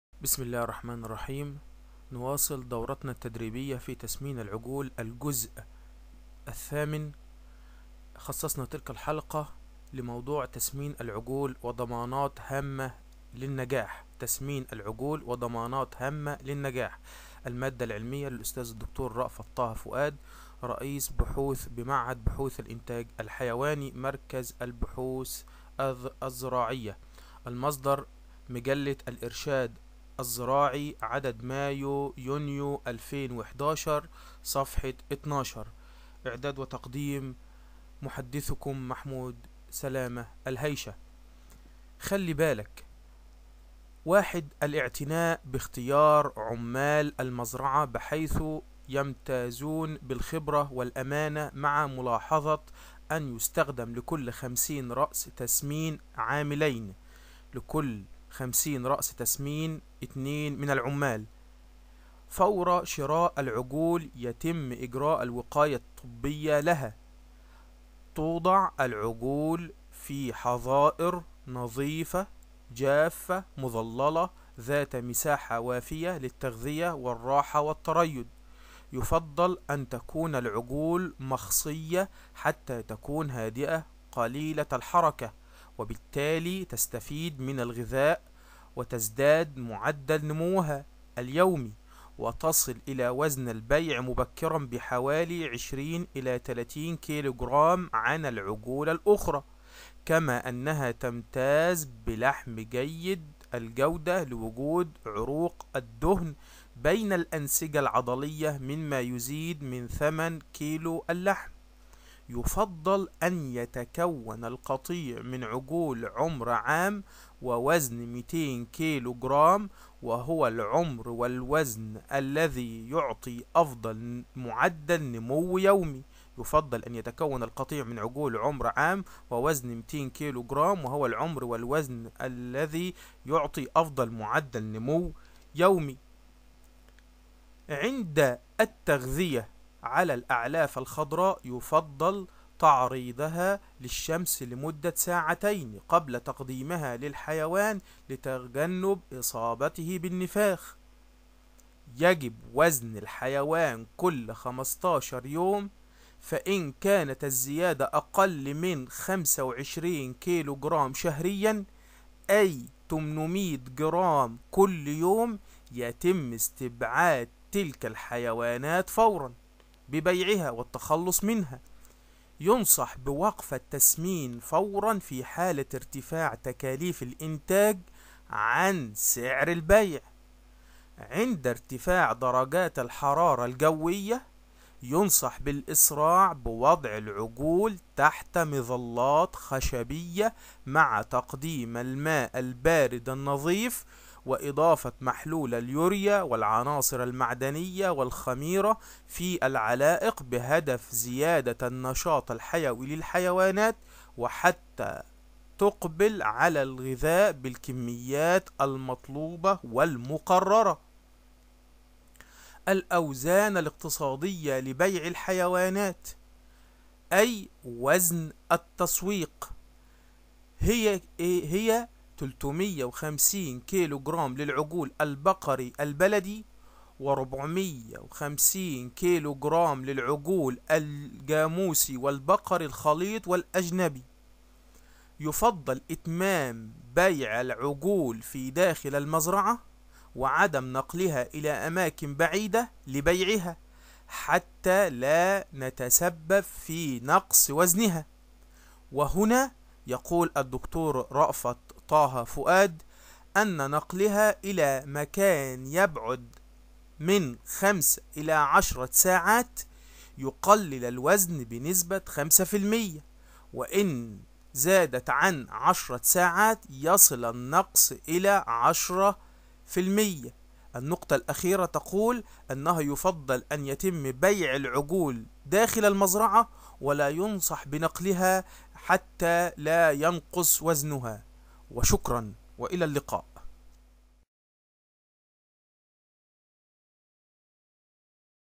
ضمانات هامة للنجاح | دورة تدريبية في التسمين | حلقة 8
هذا هو الجزء الثامن من (الدورة التدريبية في تسمين الحيوانات المجترة) وقد خصصنا تلك الحلقة عن (تسمين العجول..وضمانات عامة للنجاح)، المادة العلمية: للأستاذ الدكتور/ رأفت طه فؤاد-رئيس بحوث بمعهد بحوث الإنتاج الحيواني-مركز البحوث الزراعية (المصدر: م.عادل دانيال-مجلة الإرشاد الزراعي، عدد مايو/يونيو 2011، ص12)،  إعداد وتقديم/ محمود سلامة الهايشة.